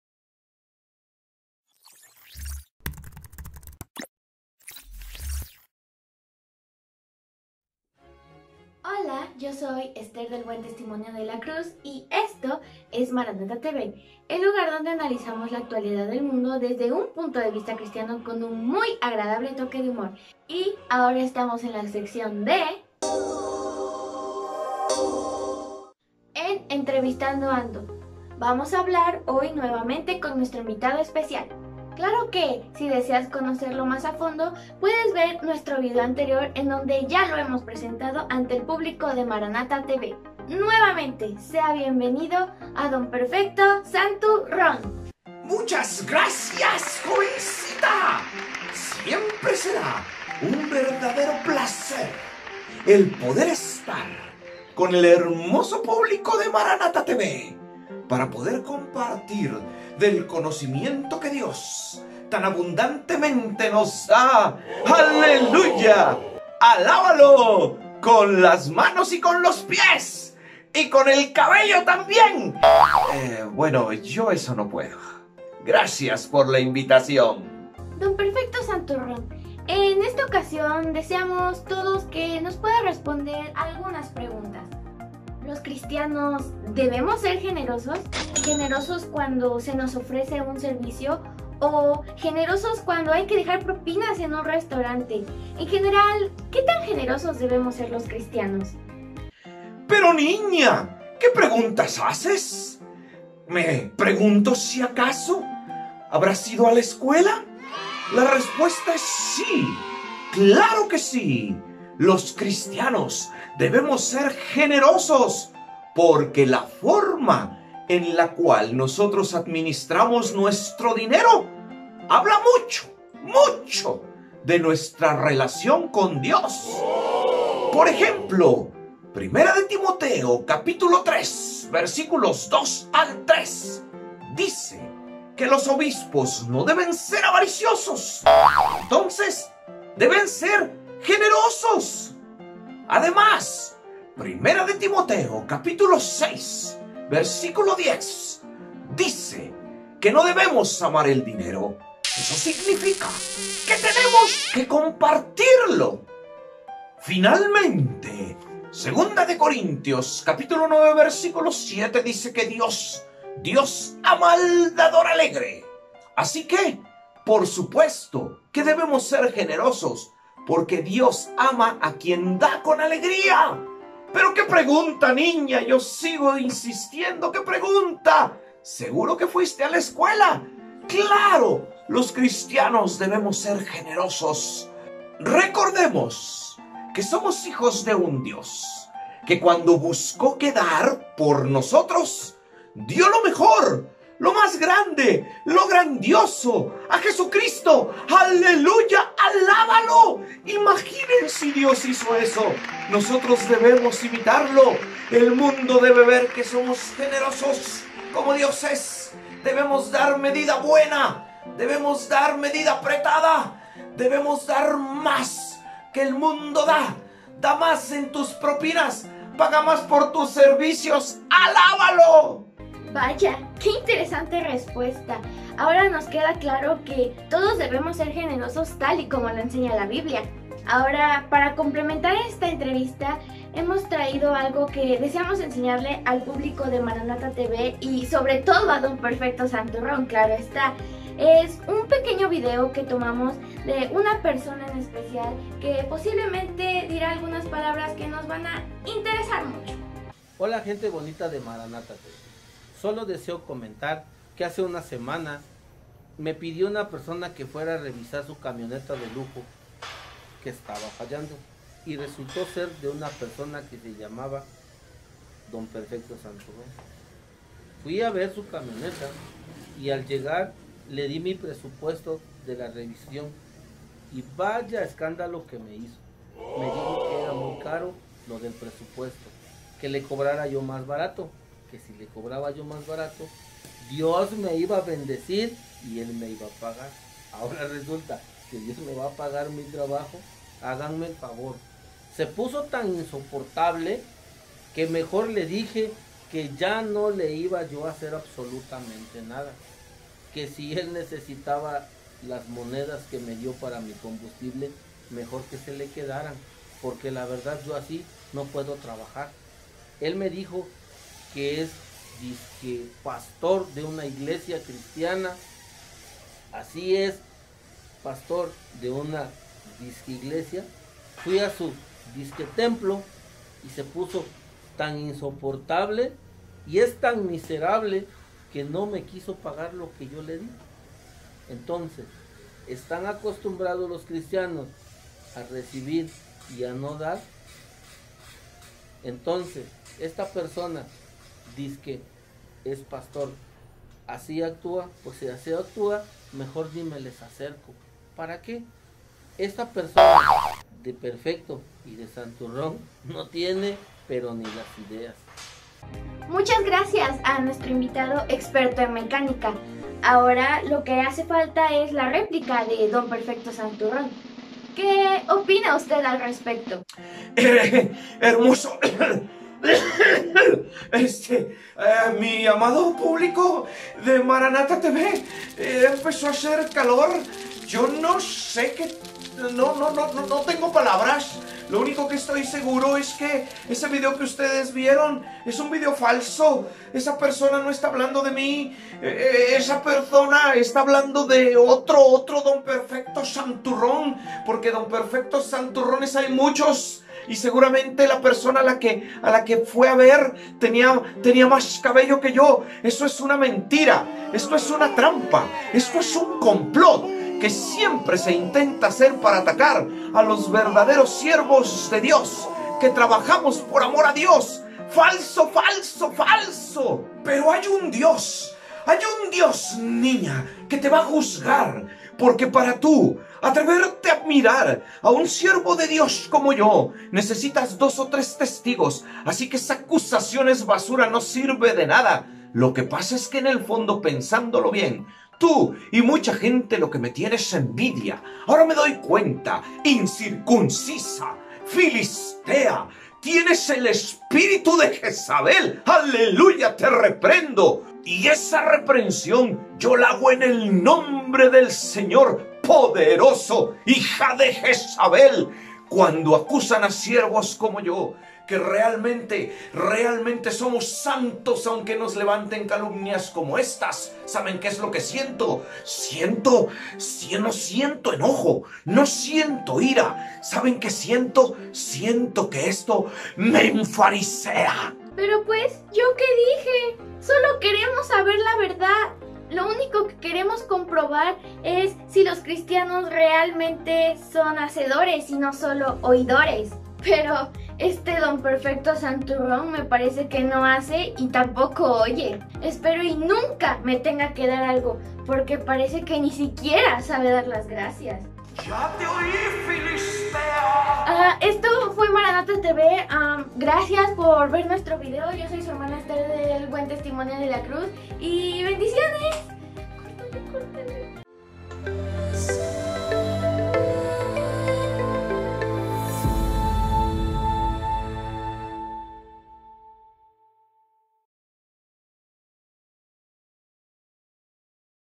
Don Perfecto Santurrón 1. La generosidad cristiana
Don Perfecto Santurrón
La generosidad cristiana

Entrevistamos a nuestro teólogo de cabecera para aprender sobre la generosidad. ¿Debemos dar los cristianos más de lo que debemos? ¿En algún servicio, en alguna propina? Pero, sobre todo, al respecto de la generosidad los cristianos ¿somos solo oídores o también hacedores?

Detalle: Por un fallo en un segmento de vídeo, por algunos minutos mostramos al inicio una imagen fija de Don Perfecto Santu Rron